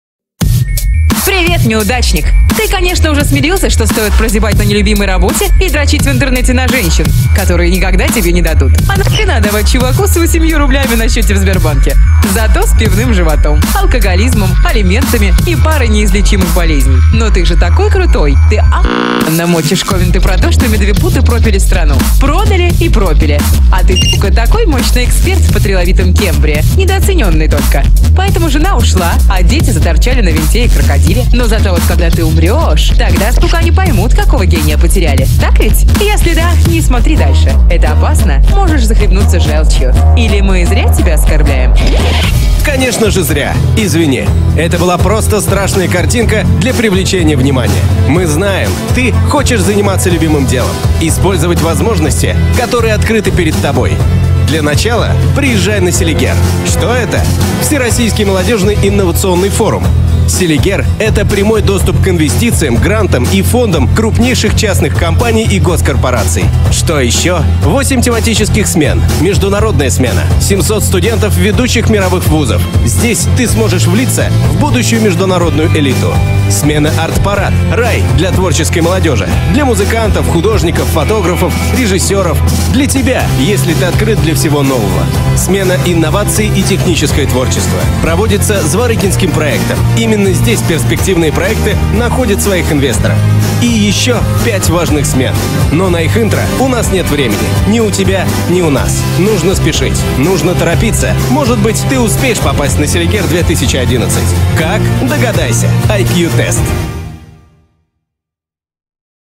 Молодежный форум будущих лидеров Селигер 2011 | danilidi.ru
Приезжайте на Селигер в 2011 году. Ожидается интересная программа. Подавайте заявки - может повезет попасть в молодежный лагерь для подготовки будущих лидеров и управленцев Надо остановить 3-ю Мировую Войну